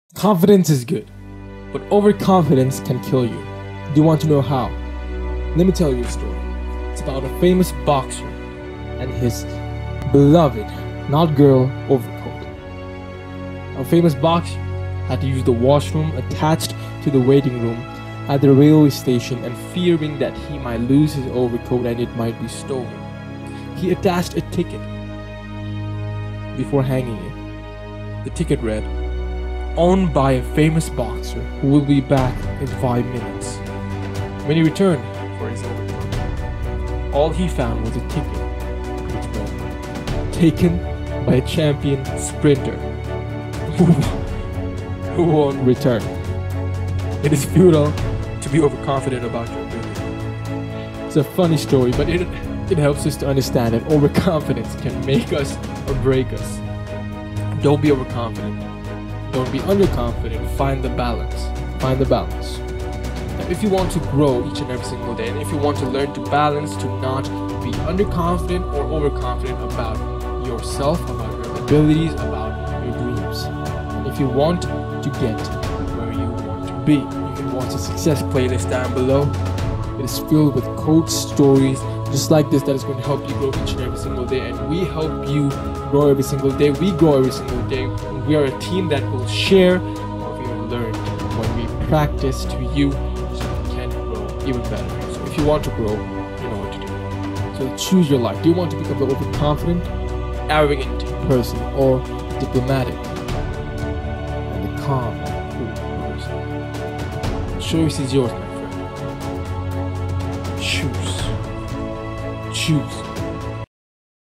OverConfidence - Boxer (don't be a fool like this)
This is a story about overconfidence about a boxer that you should hear.

It shares a story and a life lesson that you should know.

If you want success, here's a great place to start!

This is The Stranger, and our motto is “From who you are to who you want to be.”

So, choose your life right now.

Lazy Bum or Boss

Prosperity or Misery.

Choose

The Stranger.